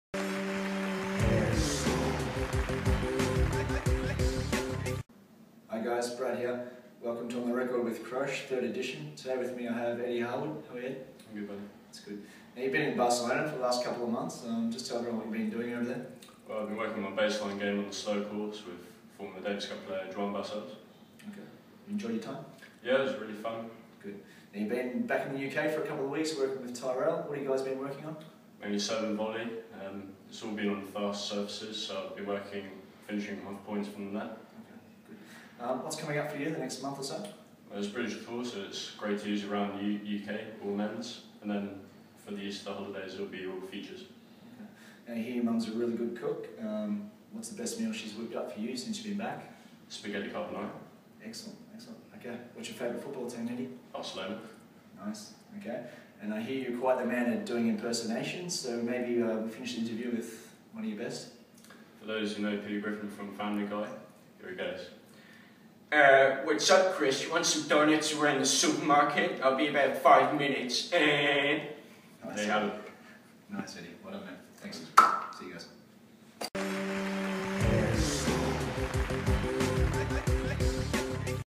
On the Record with Crosh - Edward Harwood
Edward Harwood, a full-time player at BETA, spends some time talking about the lighter side of his tennis with BETA Head Academy Coach, Brad Crosher (Crosh)!